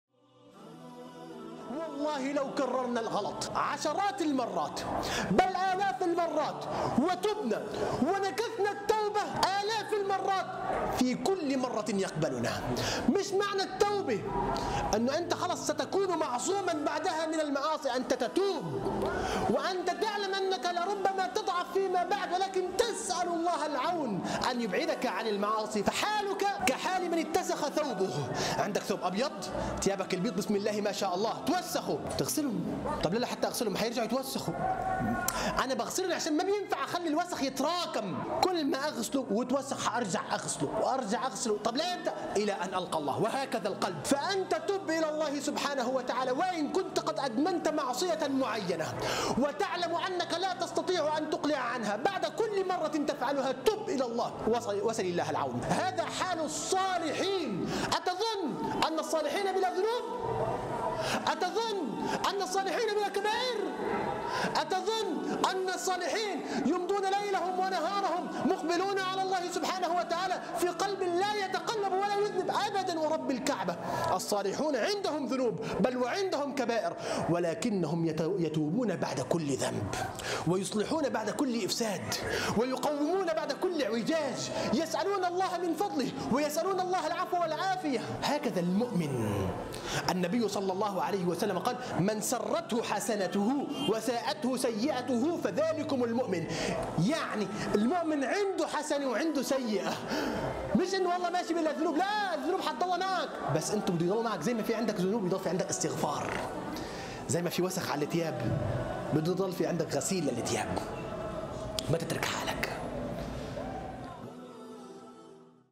أتظن أنك لست من الصالحين لأنك تذنب بعد كل توبة؟ || عبد الشكور اللبابيدي
أتظن أن الصالحين بلا ذنوب؟
أتظن أنك لست من الصالحين لأنك تذنب بعد كل توبة؟
لمشاهدة المزيد من المقاطع الدعوية للشيخ عبد_الشكور_اللبابيدي يرجى الاشتراك في
6) جديد خدمة الواتسآب 2018 : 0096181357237